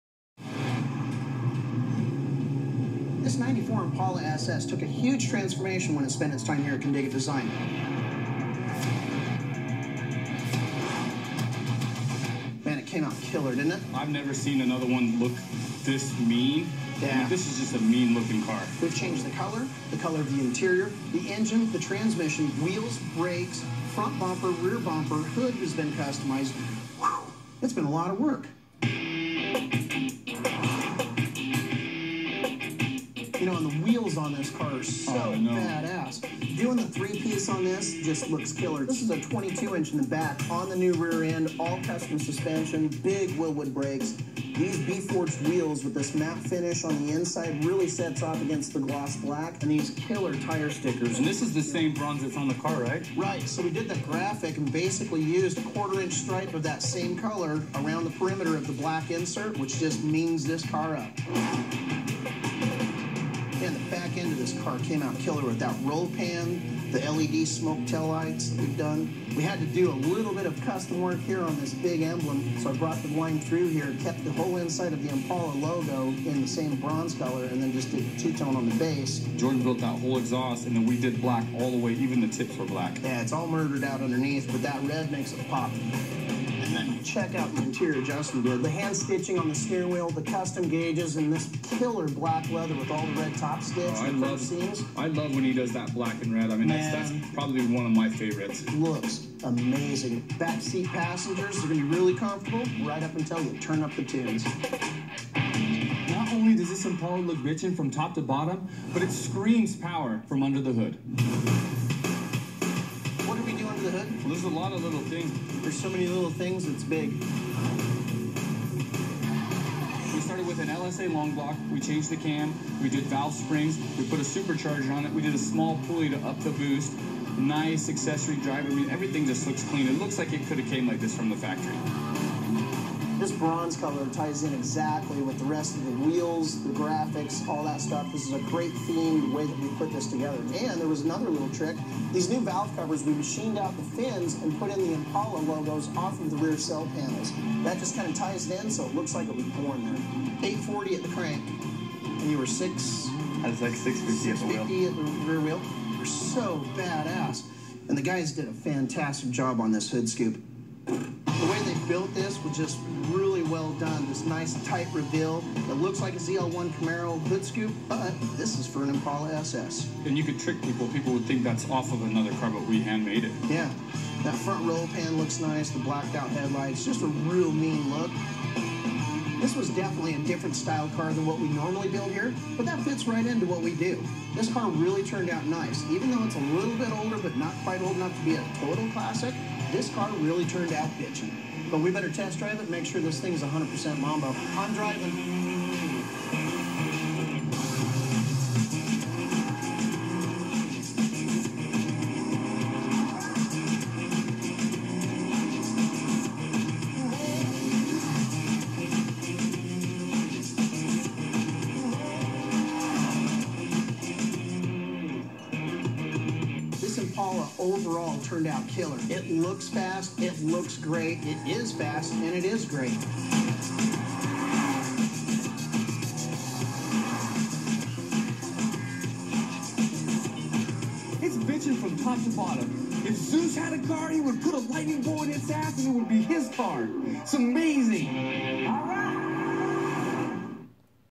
1994 Impala SS customized by Kindig-it-Design
1994 Impala SS appeared on the TV show Bitchin' Rides, lsx, magnuson supercharger, blacked out interior, bronze wheels, hood scoop.